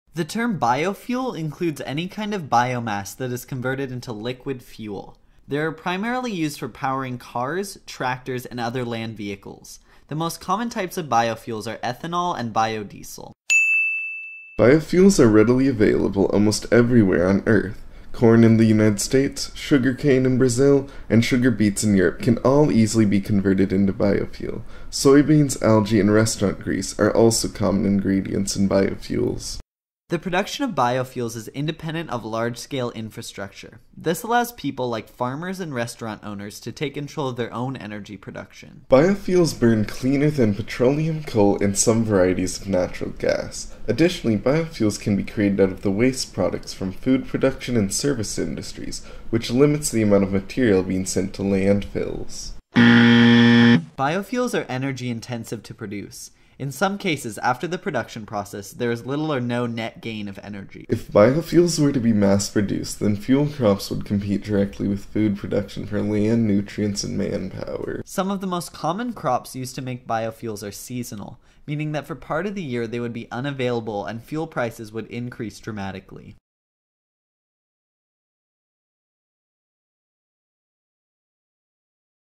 Pros and Cons of Biofuels
Pros

Readily available

Independent of large scale infrastructure

Burn cleaner than petroleum

Cons
Energy intensive to produce

Compete directly with food crops

Seasonal

Steve Jurvetson

Official U.S. Navy Page

TheFriendlyFiend

Kevin Dooley

Peter Mulligan

Sam Beebe

Justin Ruckman

Manu

Martin Fisch

Hallenser

fhgitarre

daveynin